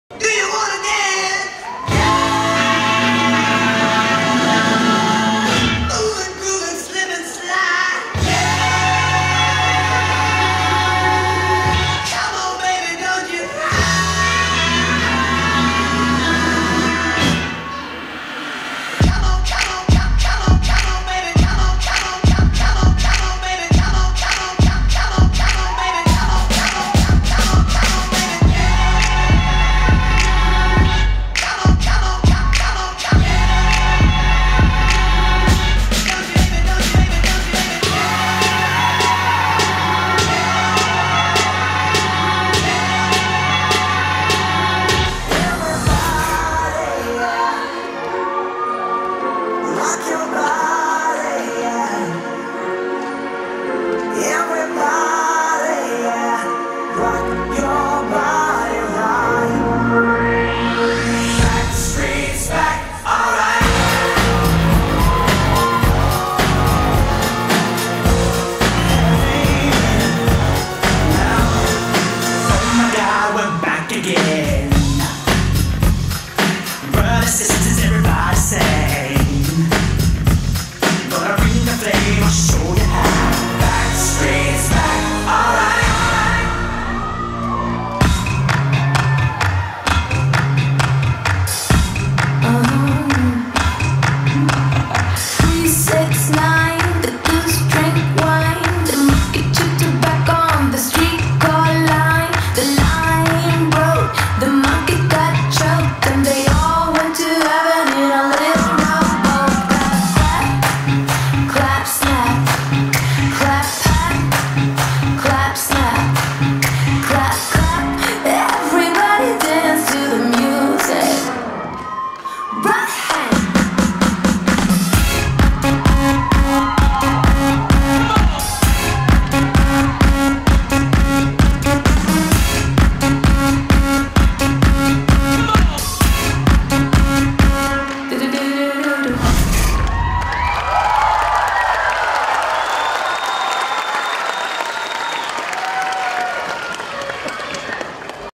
Sweeties Babies Team | Шоу Планета Active Style | Школа танцев Active Style
Школа танцев ACTIVE STYLE
Отчётное шоу ПЛАНЕТА ACTIVE STYLE
26 мая 2022, Москва

 •  Танцоры: Sweeties Babies Team
 •  Группа: Дети 5-7 лет, Хип-хоп
 •  Педагог-хореограф: Алина Гомель

🌐 Франшиза ACTIVE STYLE! Узнать подробнее

Танцуй как любишь! Пробуй бесплатно!